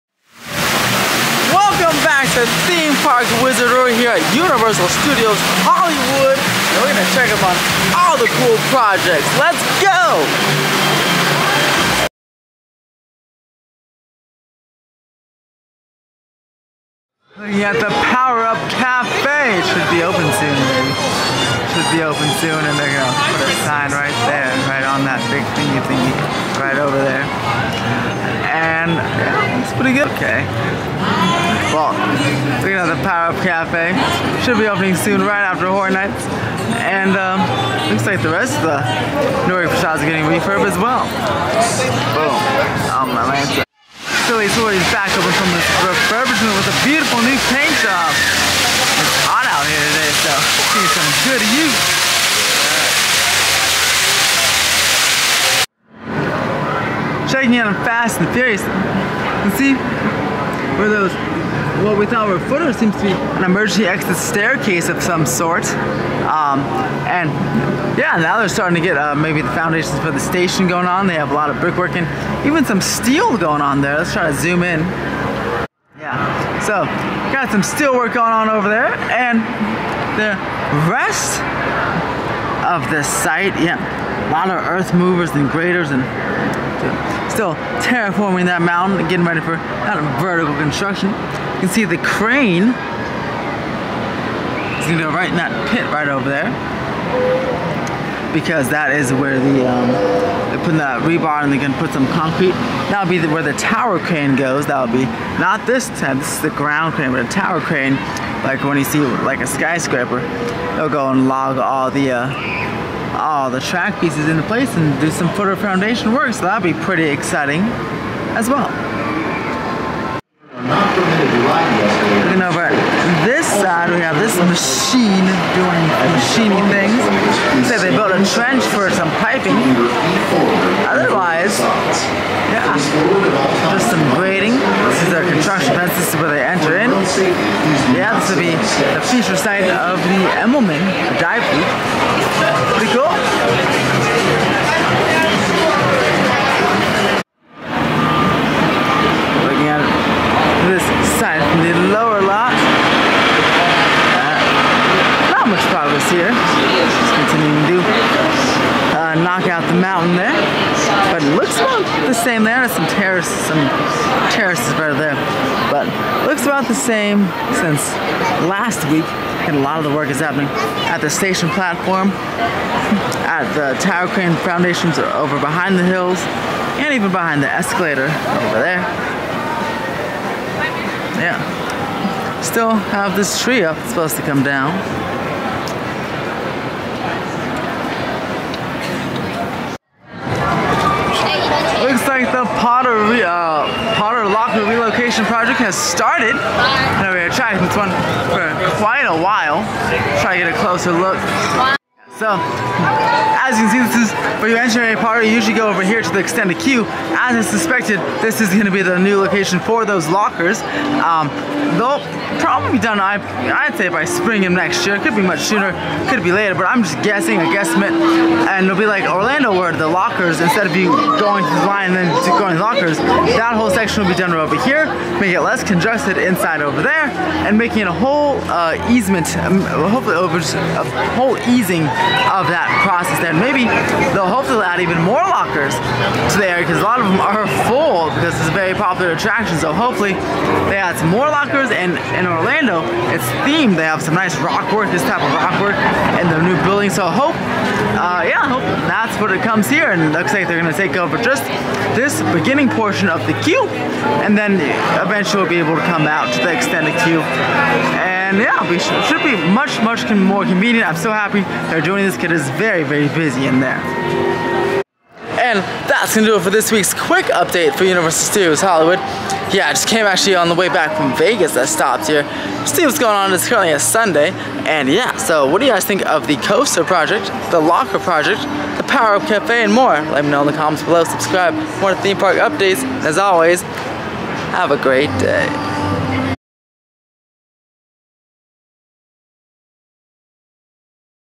New Lockers Coming To Harry Potter And The Forbidden Journey! | Universal Studios Hollywood
The new locker project has started at universalstudioshollywood , plus updates on Fast and Furious and the Power Up Cafe!